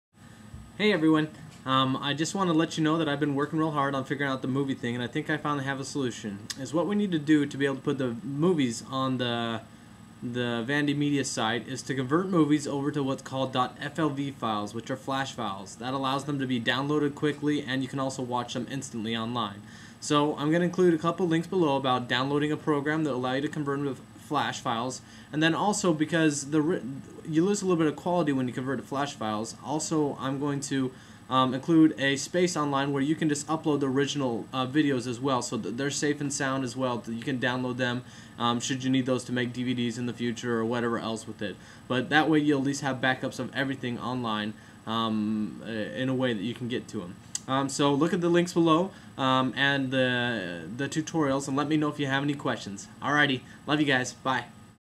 VanDMedia Video
Instructions for uploading Video to VanDMedia Site